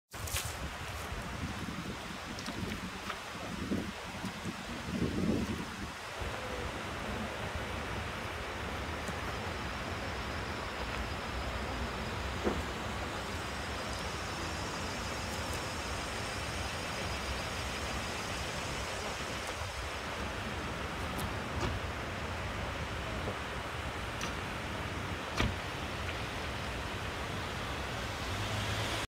GWにいかが？ 信州伊那谷隠れたハナモモの名所とは
信州伊那谷はこれから、花の季節。

こちらは駒ヶ根市　中沢のハナモモがちょうど連休中に見頃♪隠れた穴場にどうぞ♪